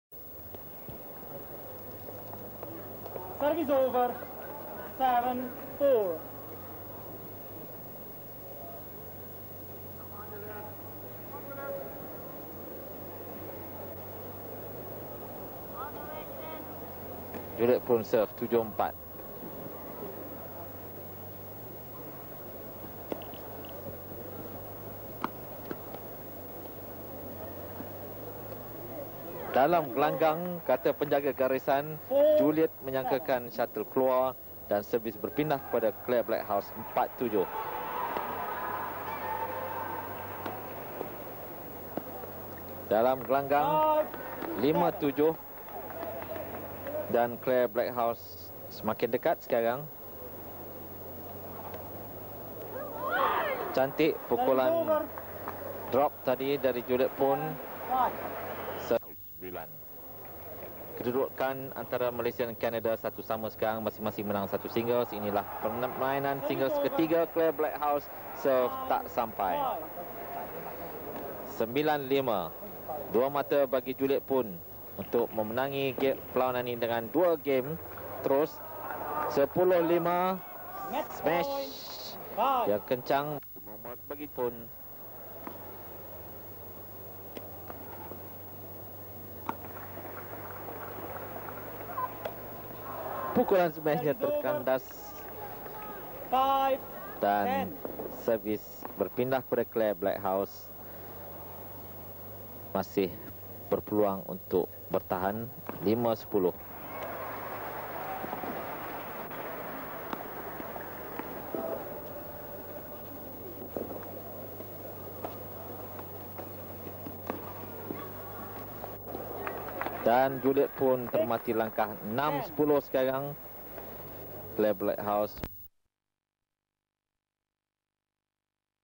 1984 Uber Cup Group A : Malaysia vs Canada (Juliet poon vs Claire Backhouse)
1984 Uber Cup Group A : Malaysia vs Canada  (Juliet poon vs Claire Backhouse)

Venue : Stadium Negara, Kuala Lumpur